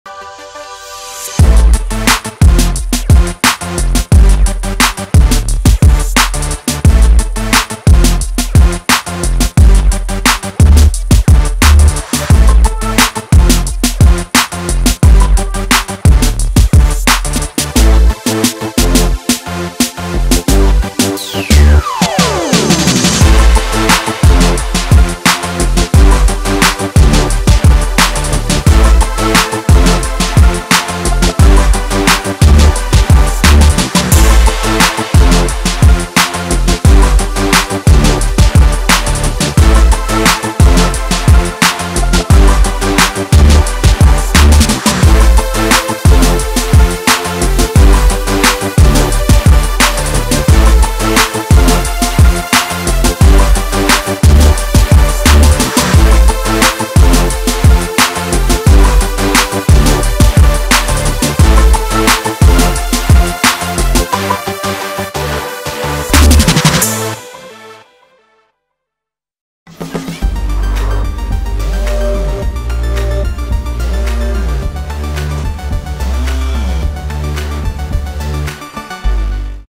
Best of Besiktas v Partizan in slow motion! | Basketball Champions League
Check out some of the best moments of the match Besiktas Sompo Japan v Partizan at gameday 2 in the Basketball Champions League.